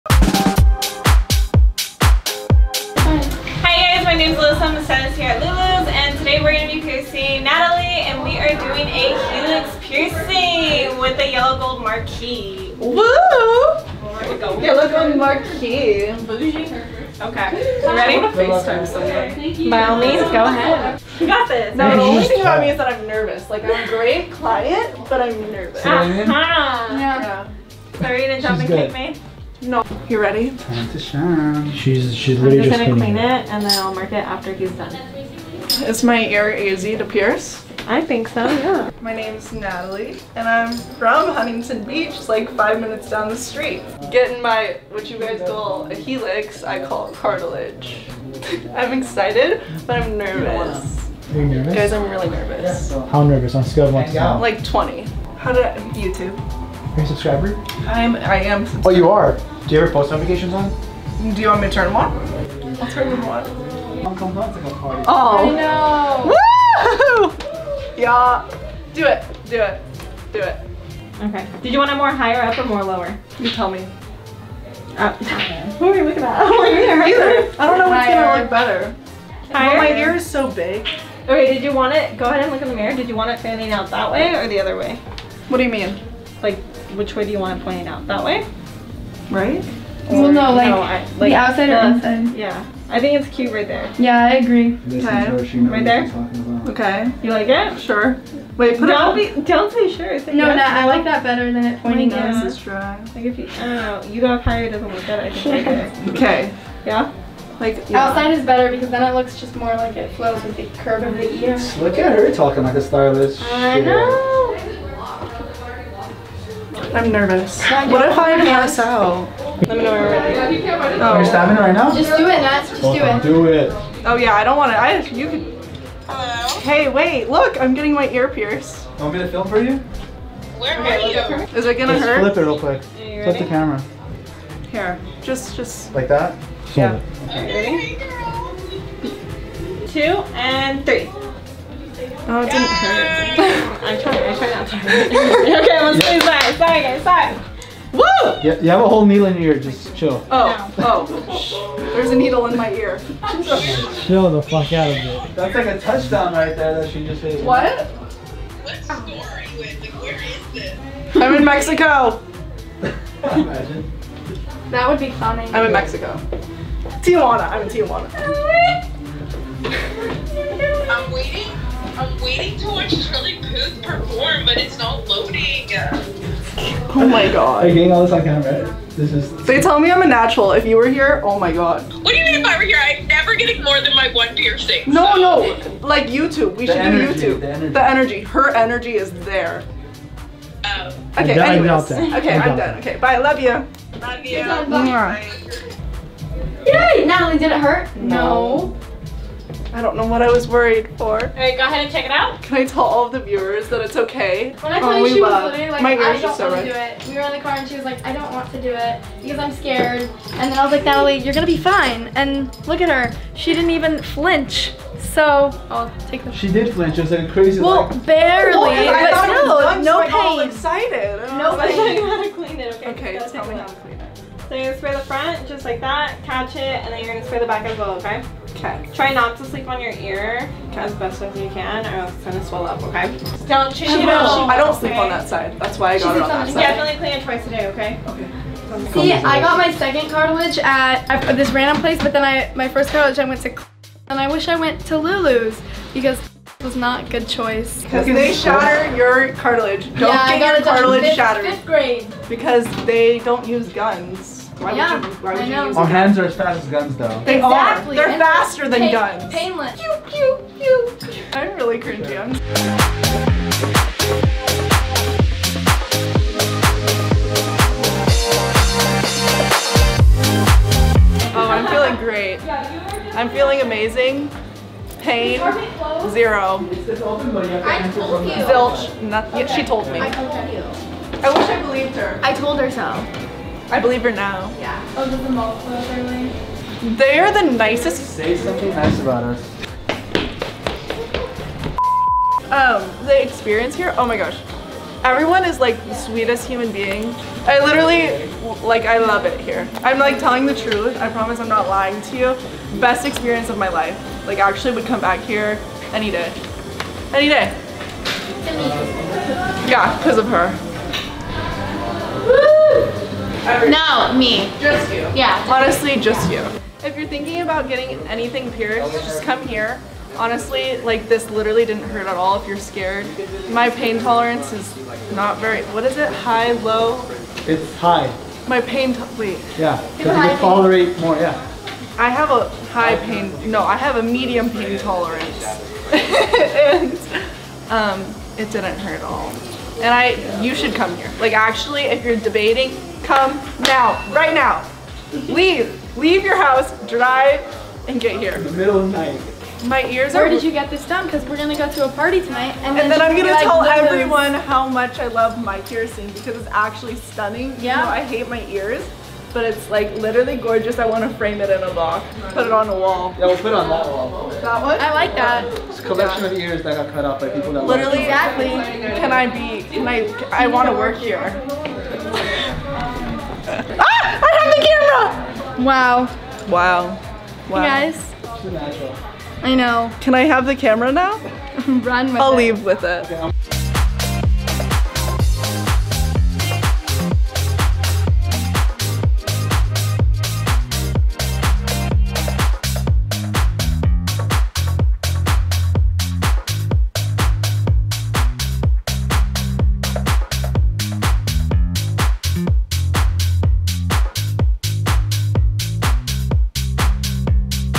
She Wanted A Job After This Piercing!
Natalie a subscriber of our channel stopped by to upgrade her look with a New Years Eve piercing. She enjoyed her experience so much she wants to know quit her job and work here?

Our Jewelry:
✨👂14KT Gold Collection